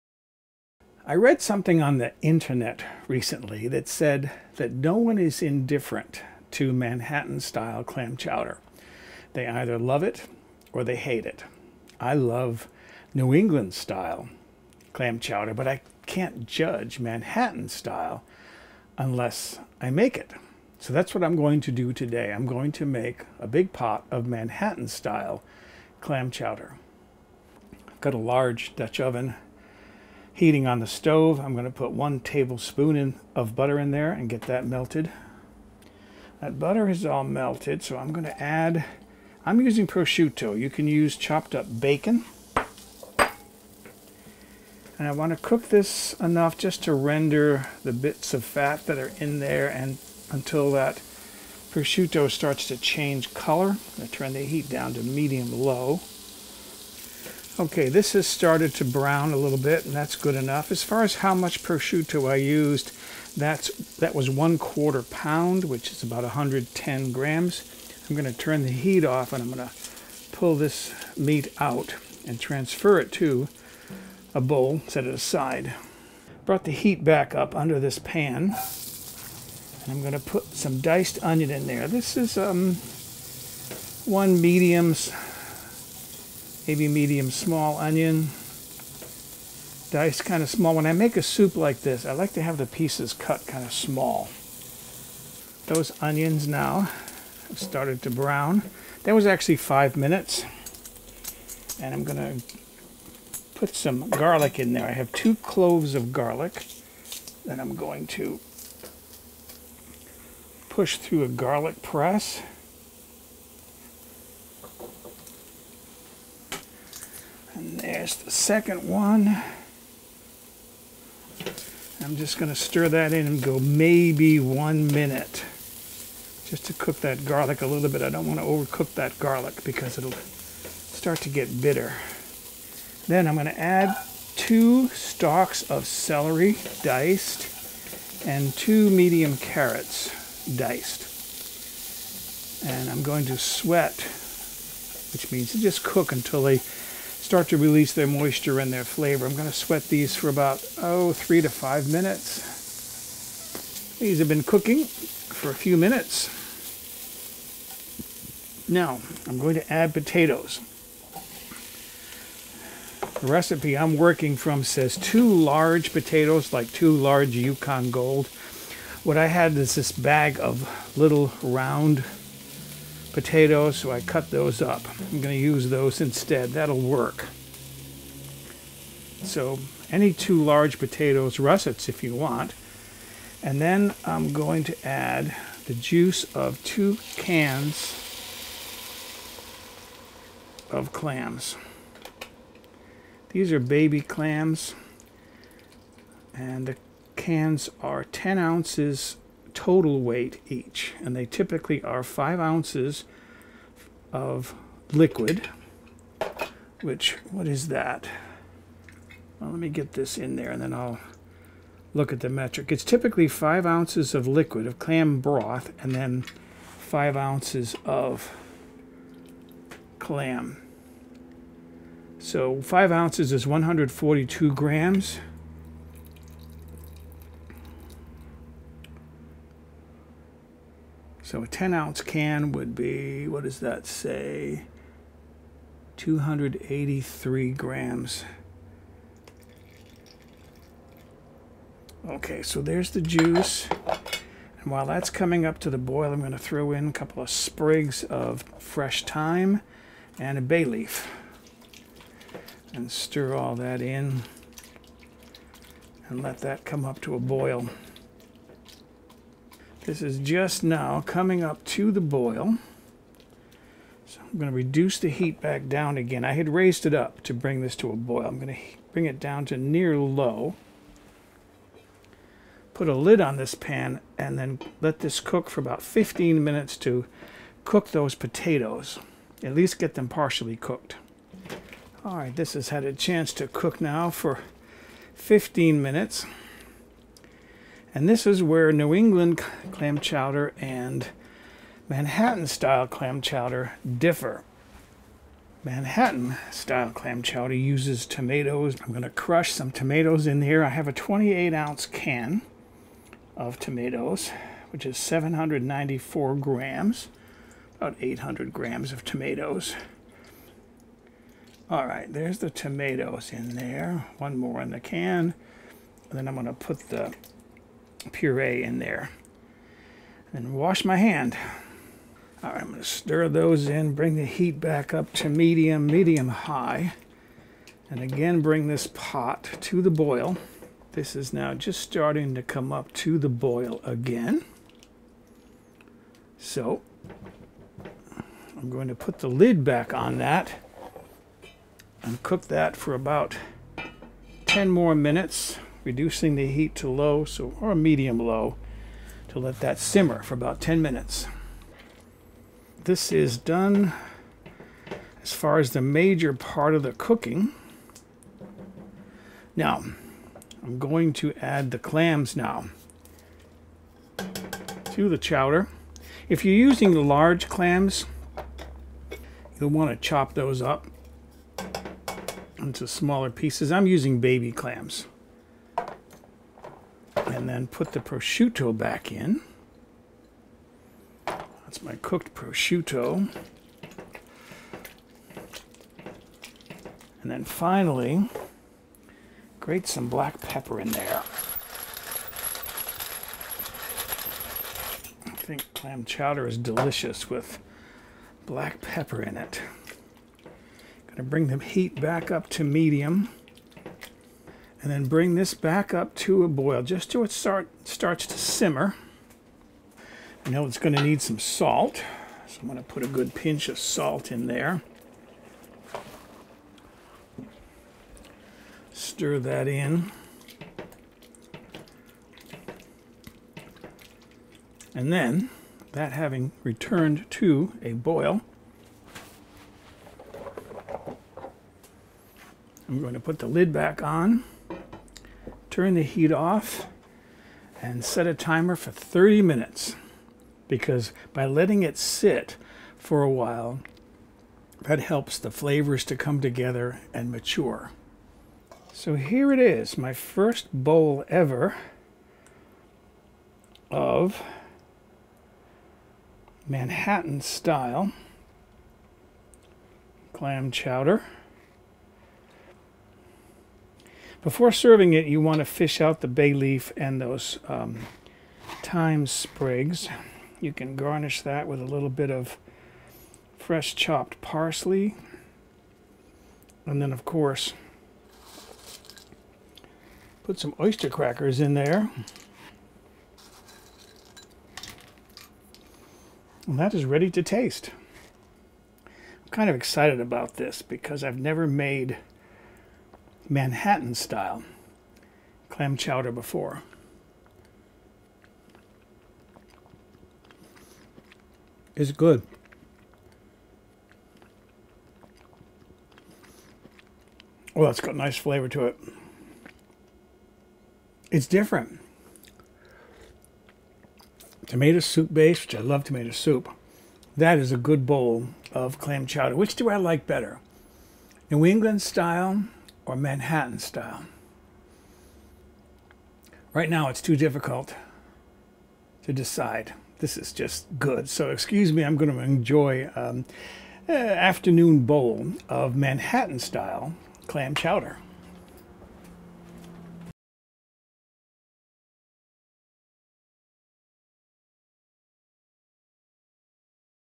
How to cook a Manhattan-Style Clam Chowder
Manhattan-style clam chowder is made with a tomato base rather than the milk/cream base found in New England clam chowder. It is a hearty soup filled with chopped vegetables, similar to a stew, and relatively easy to make. The recipe was modified from an excellent New York source — The New York Times.

And some people tell me my voice has a natural ASMR quality.